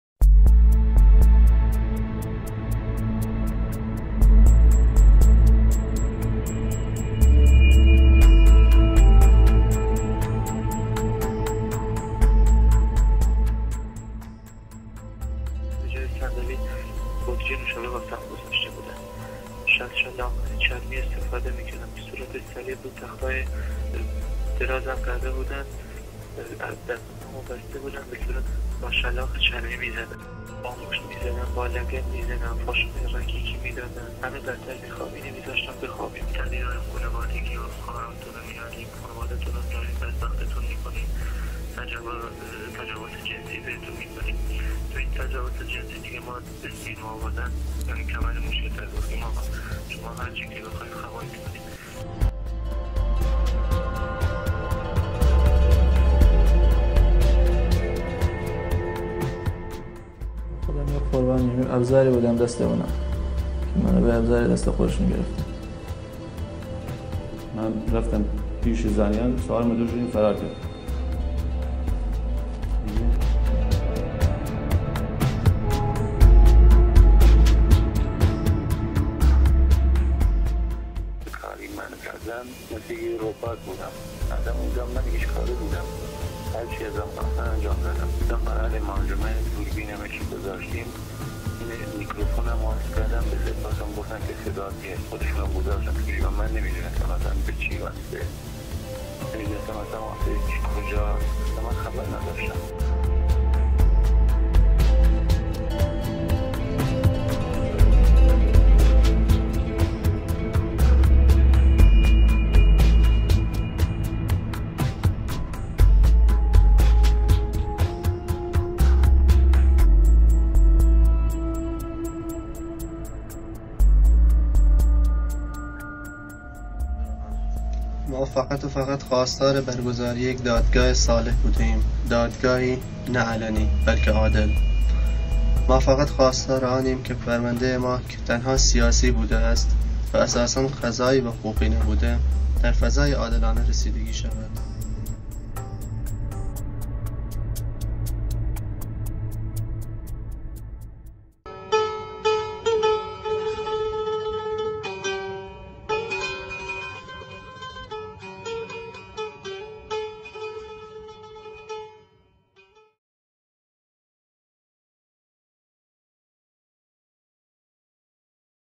طرز شکنجه و اعتراف گیری از زانیار و لقمان مرادی
از دستگیری تا اعدام، بر زانیار و لقمان مرادی چه گذشت؟